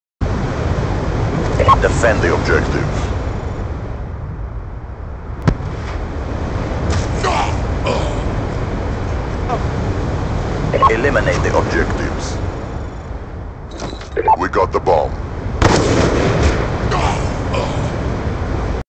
Two Crossmap Clevs in One Game!? ;)
Here we see two crossmap combat axes in one game of SnD on Hydro. Hope you enjoy!

-Best to watch in HD